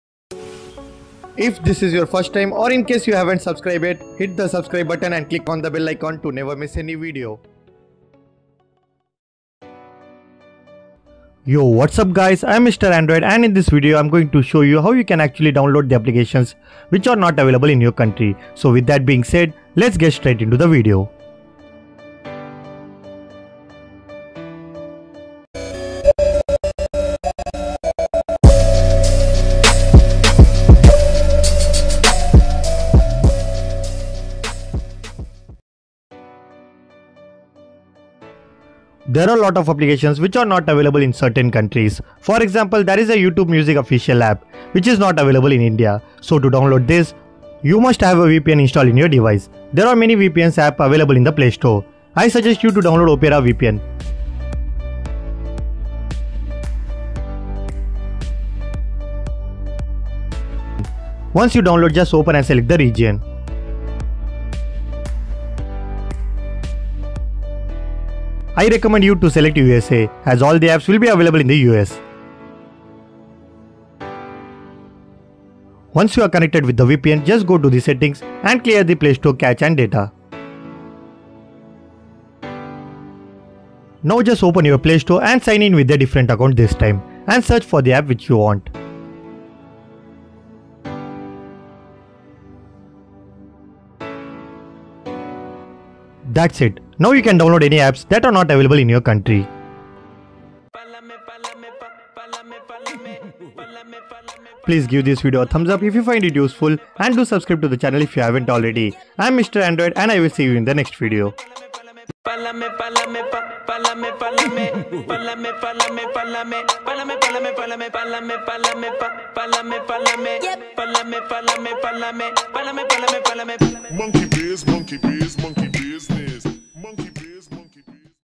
Fix - Download Apps Not Available In Your Country
Today i will show you how to fix the error, "App not available for your country" on Android Playstore and download the apps which are not available in your country.

This way , playstore will be able to search and download apps that are only limited to the US. We will have the Free Vpn app installed in the device to perform this task.

Smash The Like Button , If you find This video Useful :)

Subscription is Absolutely Freeeeeeeeee ;)

Stay Peace ;)